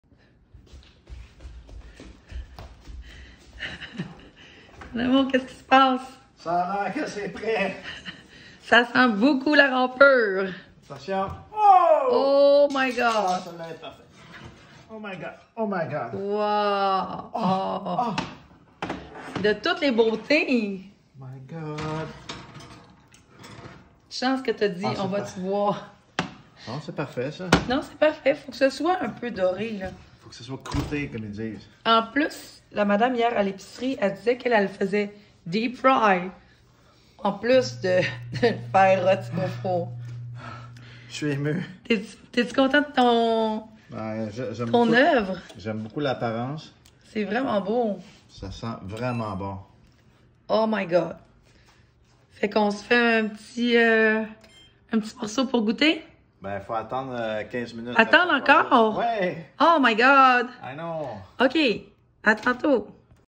La râpure est prête!
On sort la râpure du four!